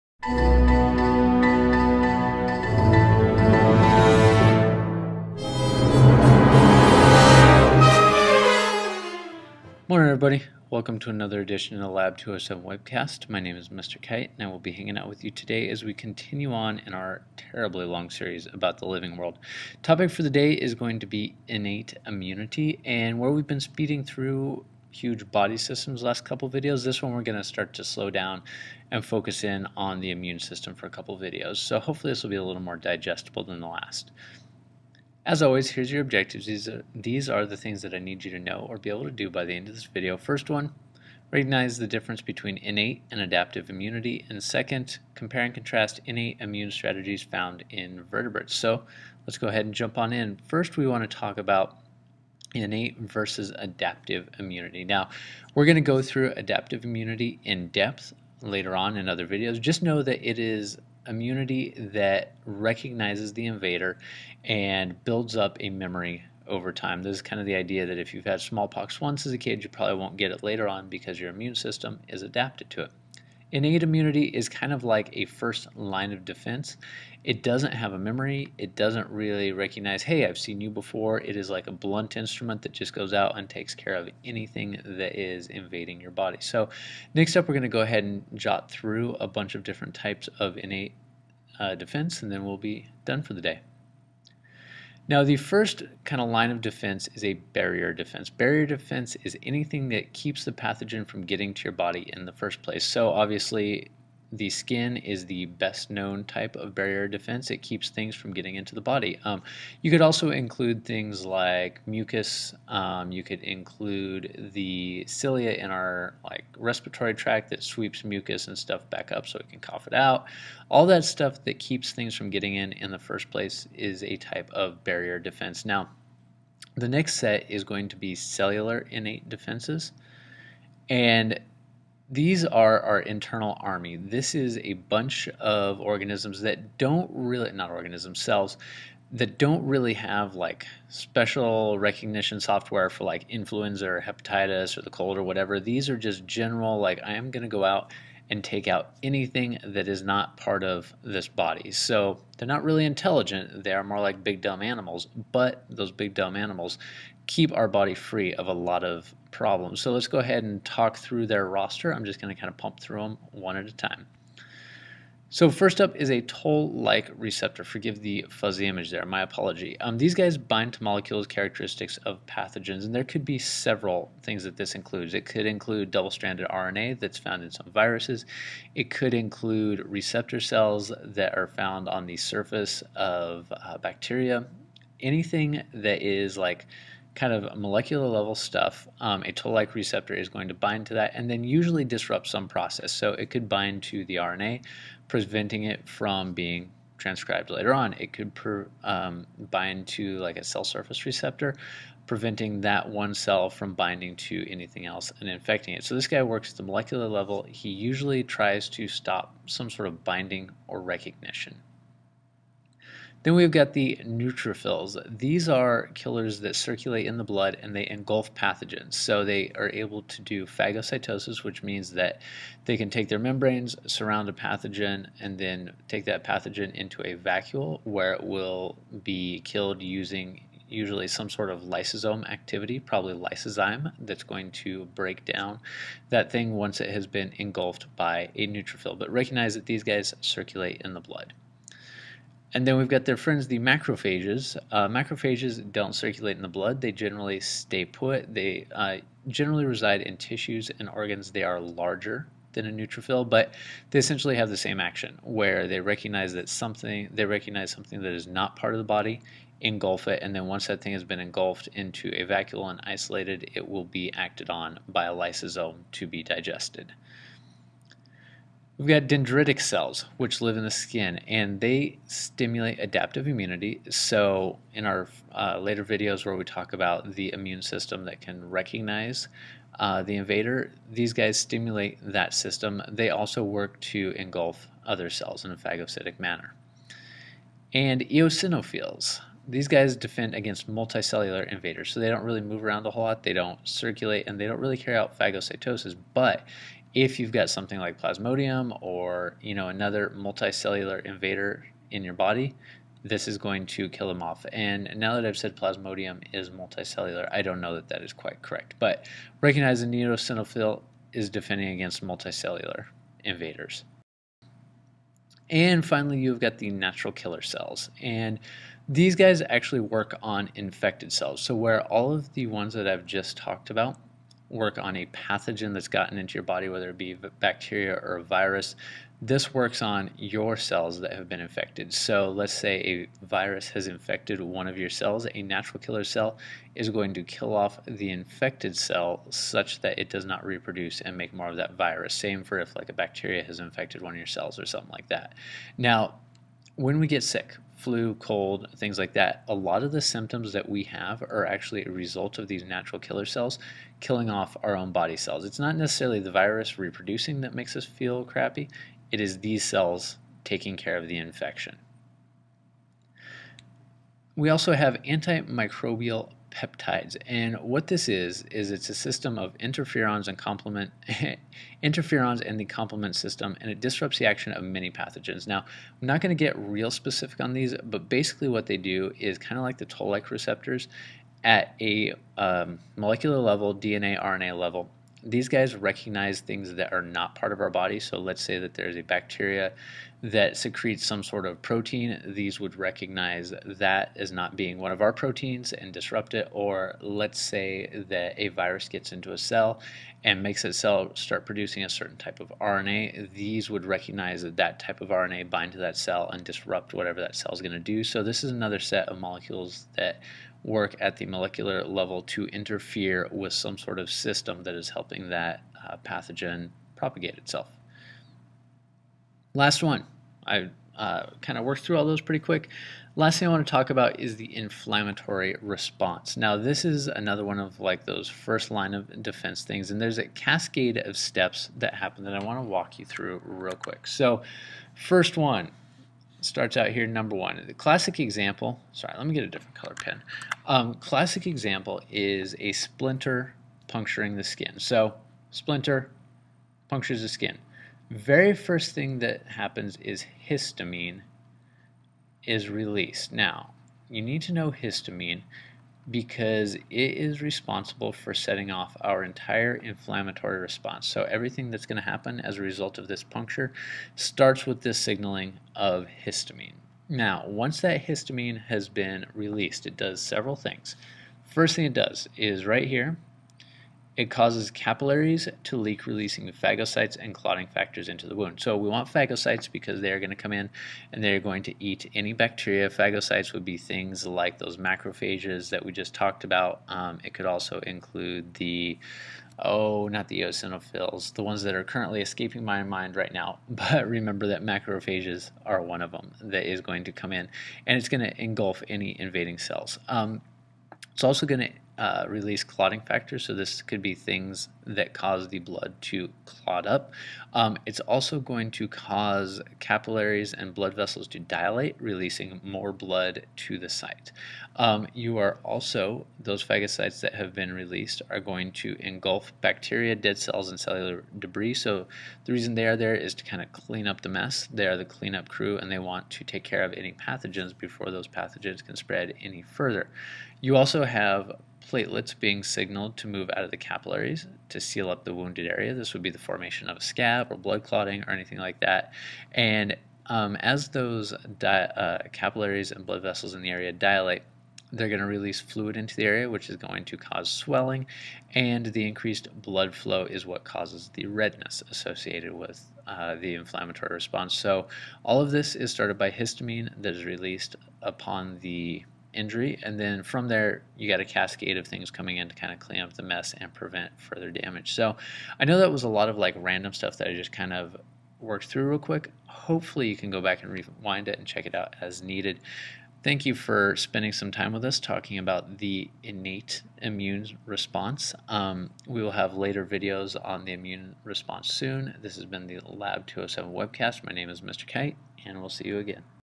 Innate Immune Response
An introduction to innate immunity.

Table of Contents:

00:35 - Objectives
00:51 - Innate vs. Adaptive
01:34 - Barrier Defenses
02:10 - Cellular Innate
02:45 - Toll-Like Receptor
03:32 - Neutrophils
04:02 - Macrophages
04:28 - Dendritic Cells
04:47 - Eosinophils
05:14 - Natural Killers
06:03 - Antimicrobial Peptides
06:58 - Inflammatory response